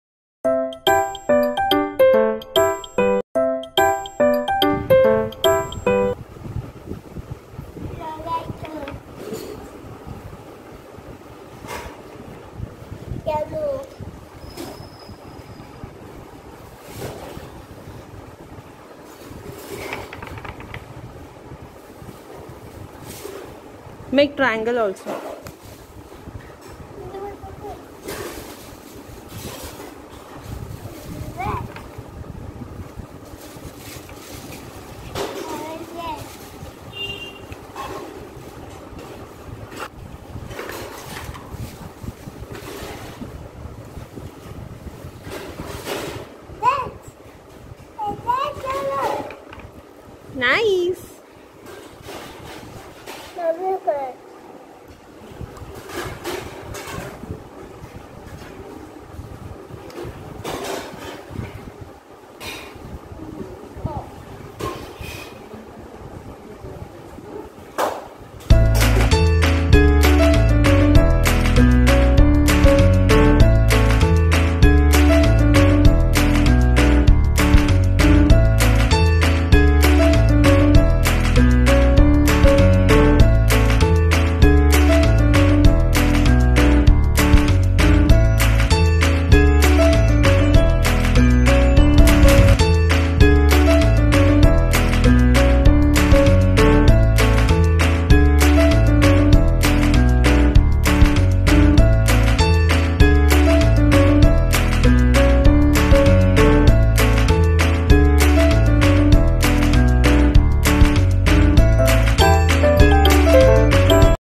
Painted my own shirt!!
Hello!!!
As I love painting so I painted my own shirt.Do check it out !!